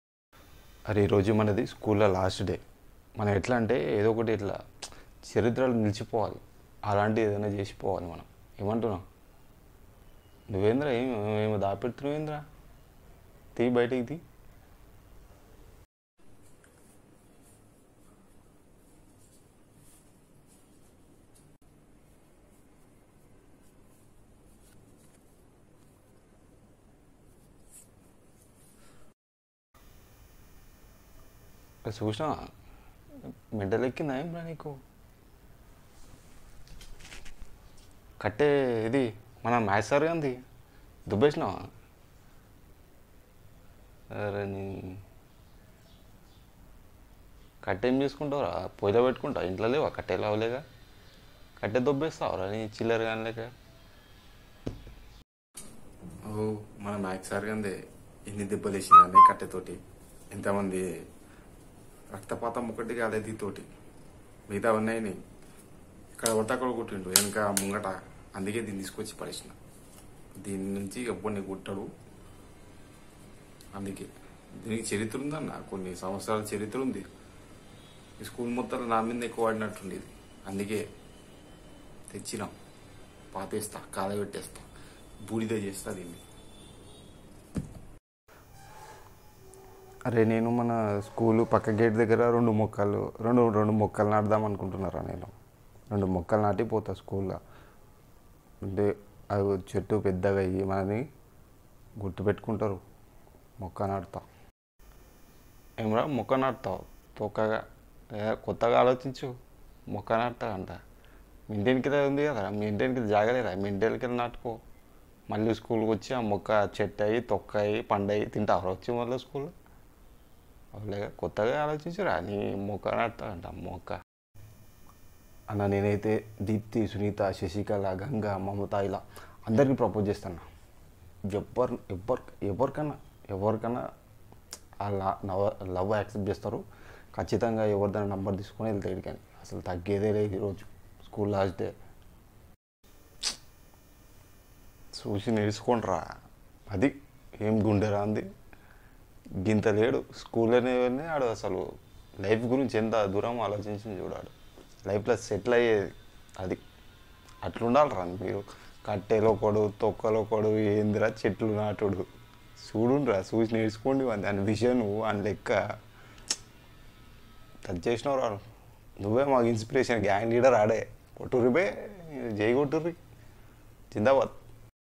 School Last day planes/ funny school friends/ Gang leader, Topper, lover boy, Rowdy student
Gang of students planning, what to do last day of school.

Gang leader is asking suggestions.
Topper want to plant a plant.
Rowdy student stealing his maths sir stick.
Lover boy wants to propose his girl classmates.